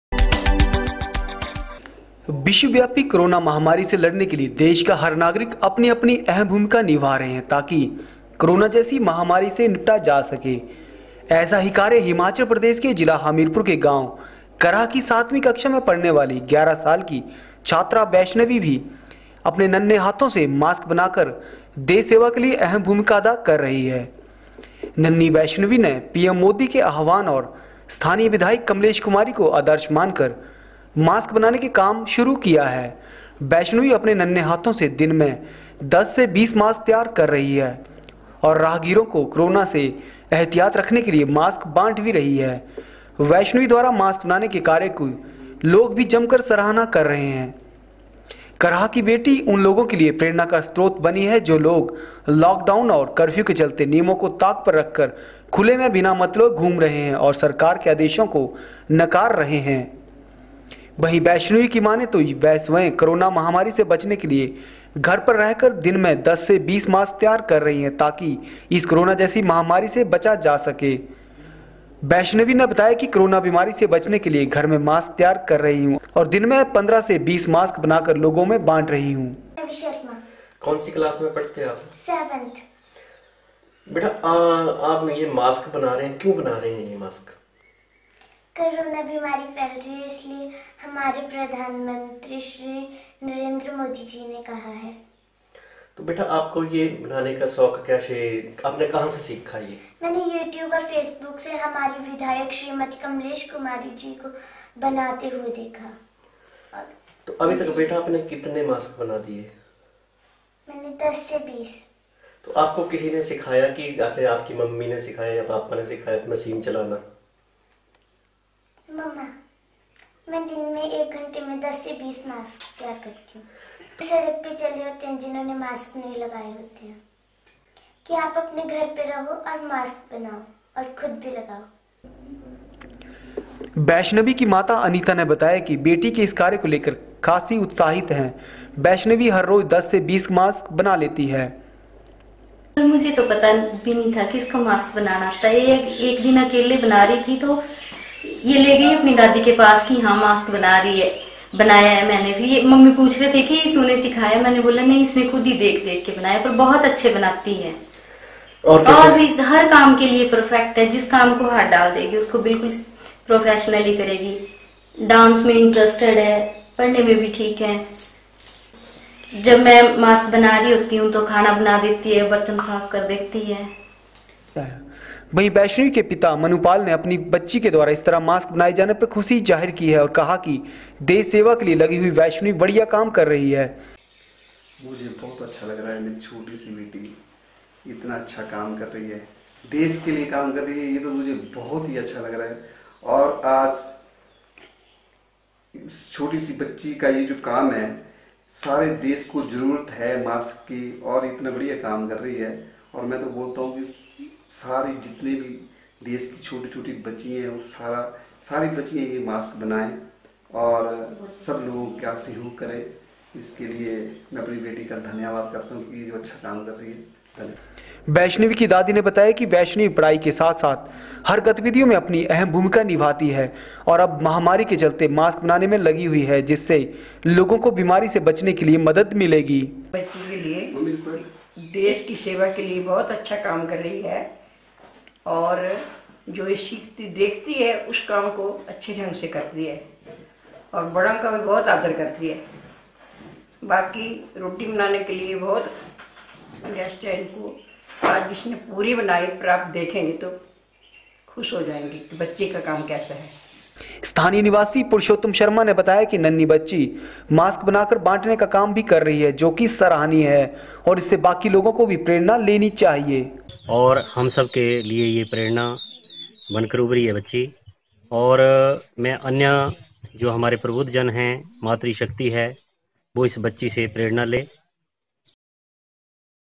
13 april 06 भोरंज के करहा गांव की 11वर्षीय बैष्णवी ने पेश की मिसालए नन्हे हाथों से बना रही
हिमाचल प्रदेश के जिला हमीरपुर के गांव करहा की सातवीं कक्षा में पढ़ने वाली 11 वर्षीय बैष्णवी ने मास्क बना कर मिसाल पेश की है। विश्व व्यापी कोरोना जैसी महामारी के चलते बैष्णवी ने मास्क बनाकर  कोरोना वायरस से बचाब के प्रति लोगों को जागरूक करने में अपनी अहम भूमिका निभा रही है। नन्ही बैष्णवी ने पीएम मोदी को आदर्श मानकर अपने नन्हें हाथों से मास्क सिलकर राहगीरों को कोरोना महामारी से बचाब के लिए मुहँ पर मास्क लगाने की नसीहत दे रही है। वैष्णवी हर रोज 10 से 20 मास्क सिल रही है। बैष्णवी के इस कार्य को लेकर लोग जमकर तारीफ कर रहे हैं।